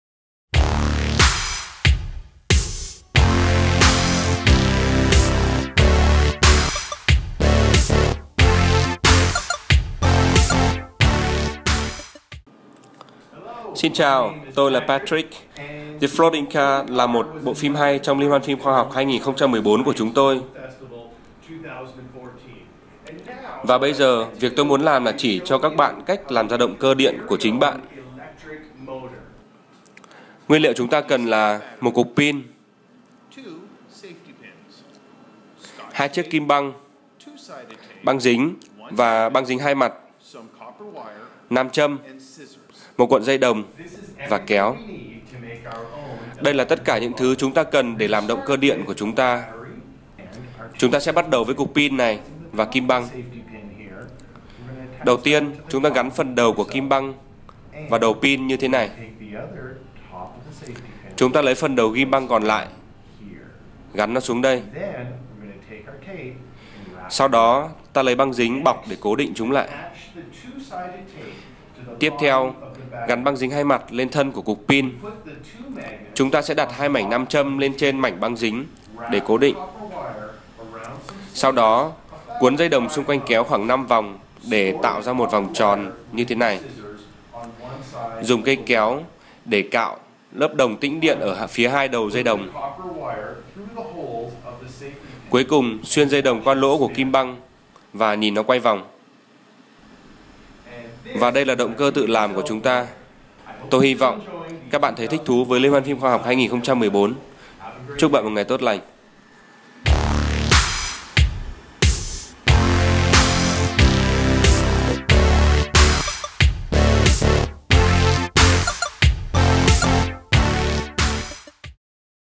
DIY Motor Final
Bạn đã từng làm một chiếc motor quay tít mù chỉ với một viên pin, nam châm và cuộn dây tự cuốn chưa, còn hơn cả một thí nghiệm, nó thật sự là một phép màu quanh chúng ta.